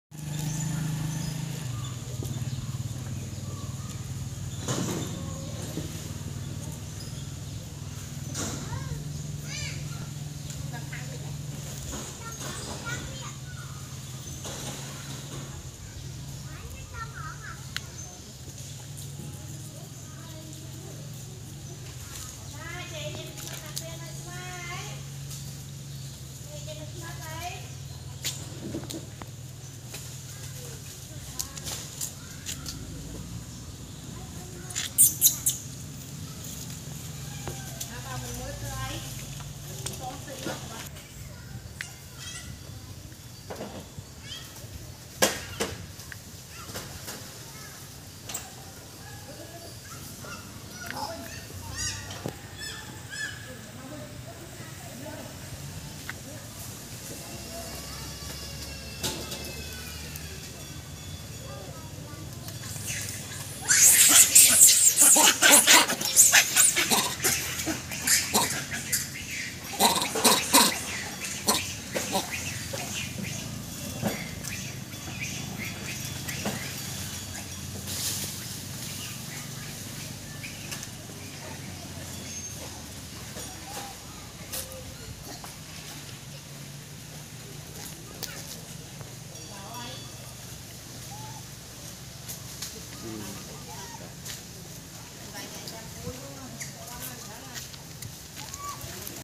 Mom always near you
In this video is showing you about mom and a new baby's life.